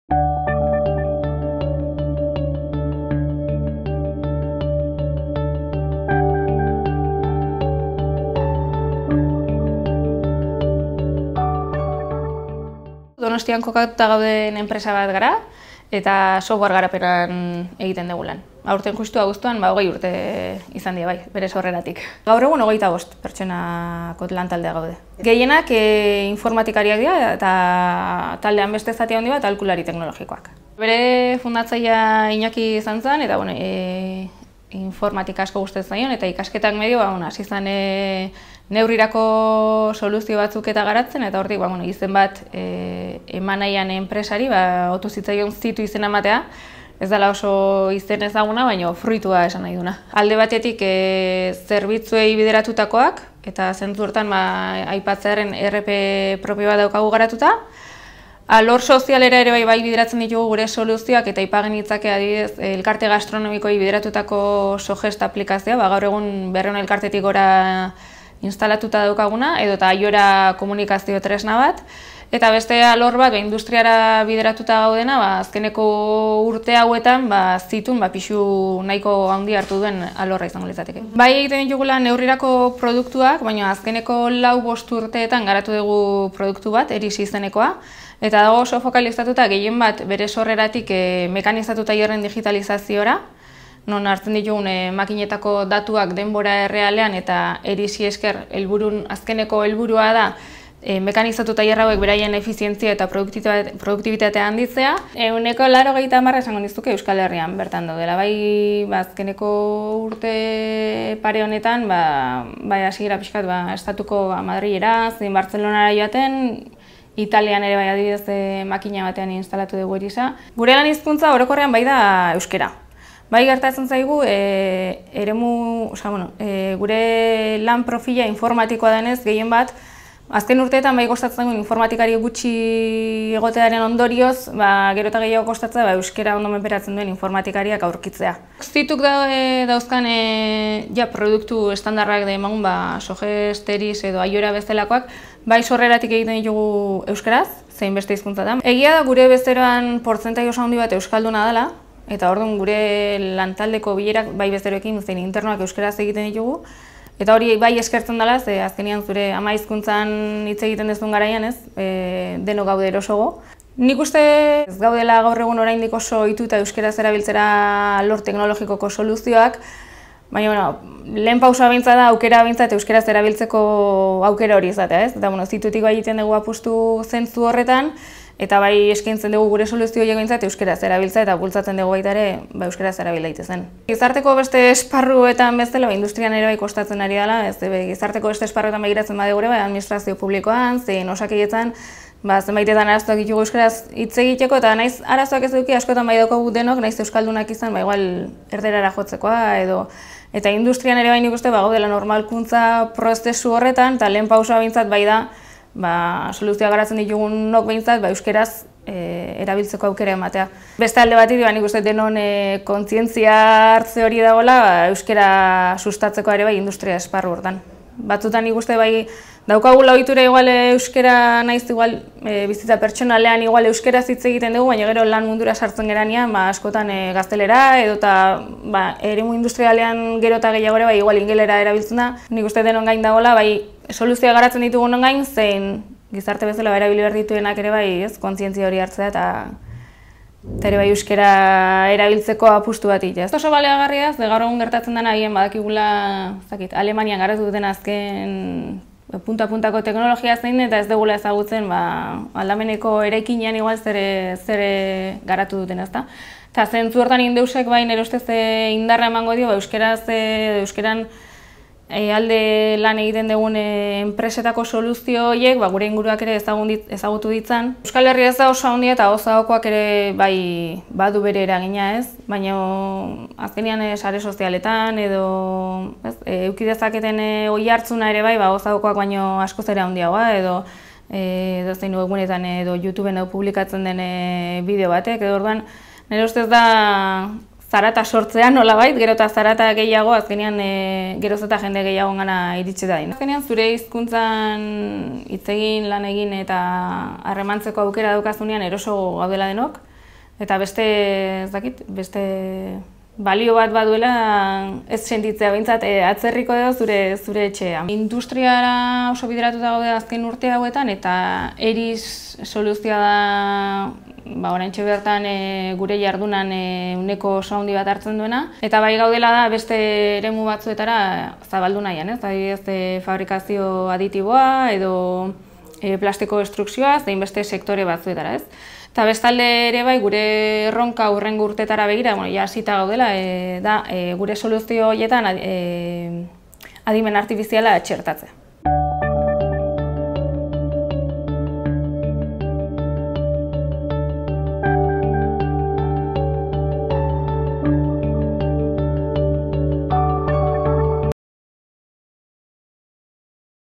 “Gure hizkuntzan hitz egiteko aukera daukagunean erosoago gaude denok”
Orain dela 20 urte Donostian sortutako enpresa da ZITU bezeroei neurrira egindako soluzio teknologikoak eskaintzen dizkiona eta bere produktuetan euskara txertatzen duena.  Nekane Ubillosekin egindako solasaldiak eman digu ZITU bertatik bertara ezagutzeko aukera.

INDEUS, Industriaren Euskal Plataforma industrian euskara bultzatzeko sortu zen eta euskararen presentzia areagotuz, euskal enpresen lehiakortasunaren euskarri izatea du helburu.  Gerturatu webgunera INDEUS ezagutzeko.